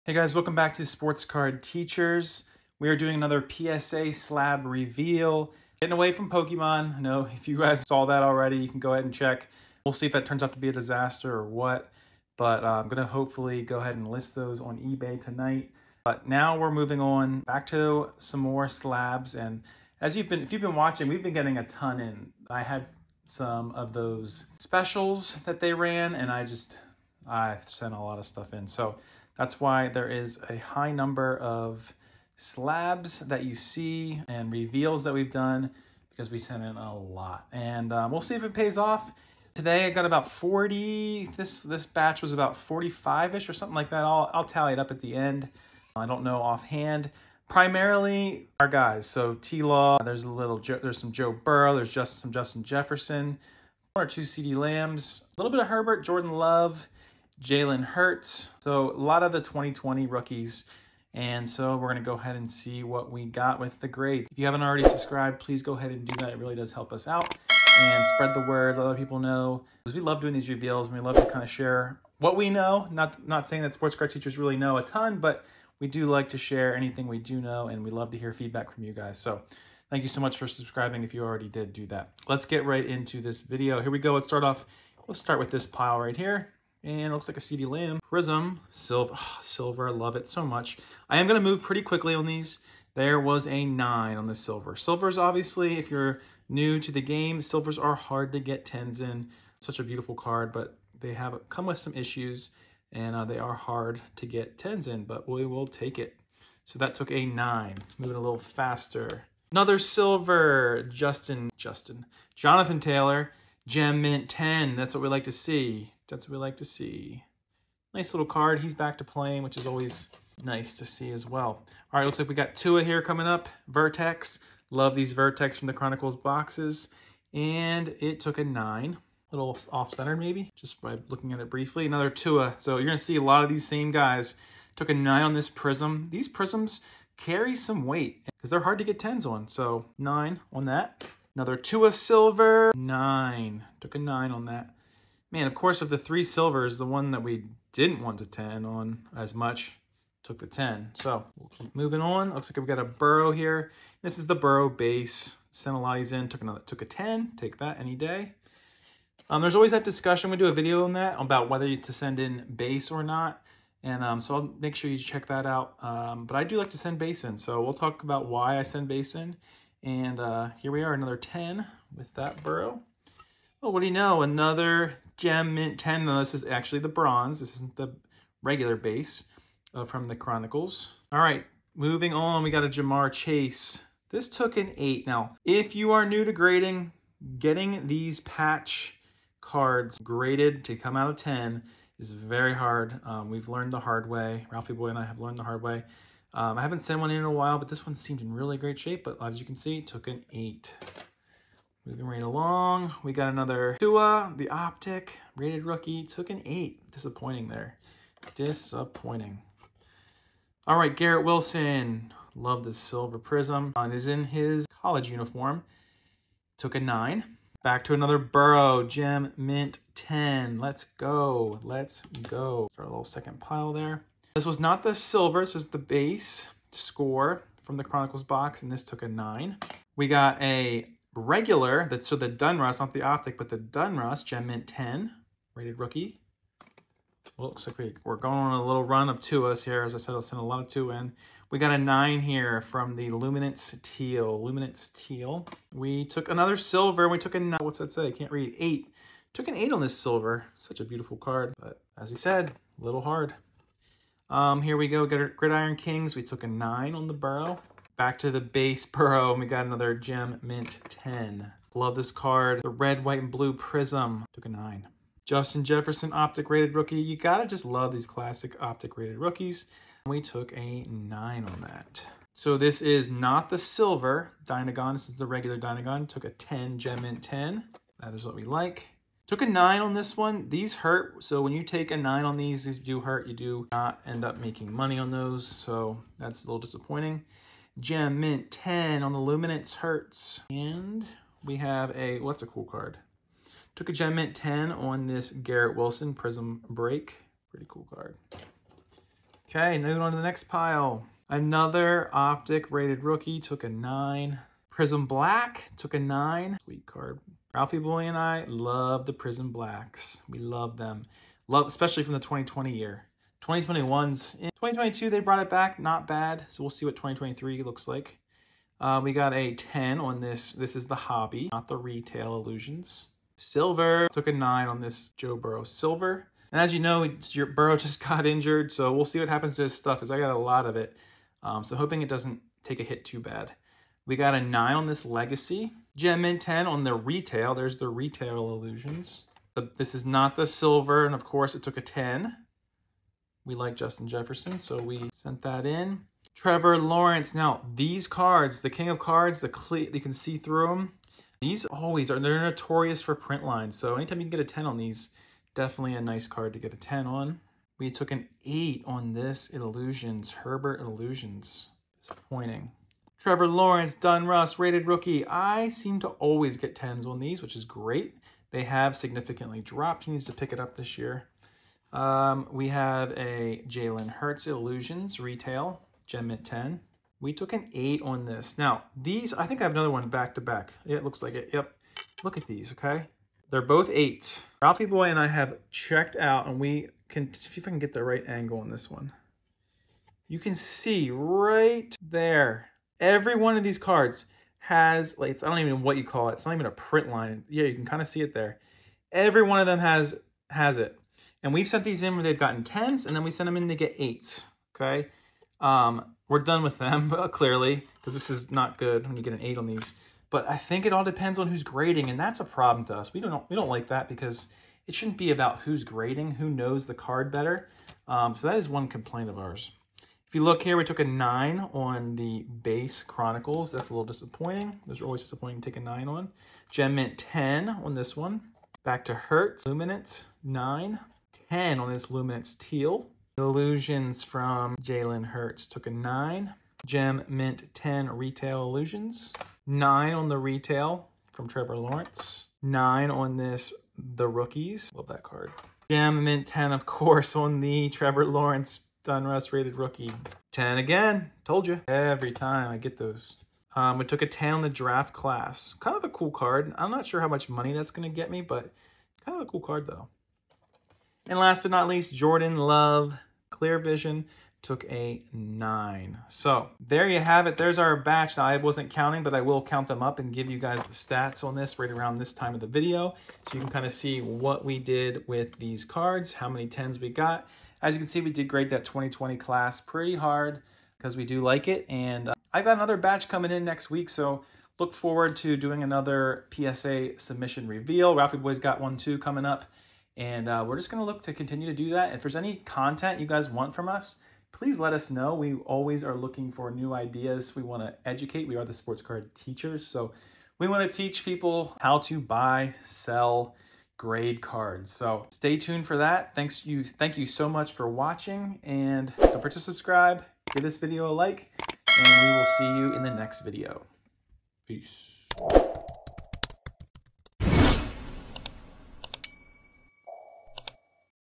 PSA Submission Reveal - Grading Hurts, Tagavailoa, Burrow, Jefferson & Much More!
In this video Sean reveals his latest batch of PSA Football Card Submissions.  In this batch the goal is as many Gem Mint PSA 10s of players like Joe Burrow, Tua Tagavailoa, Jalen Hurts, Justin Jefferson and much more.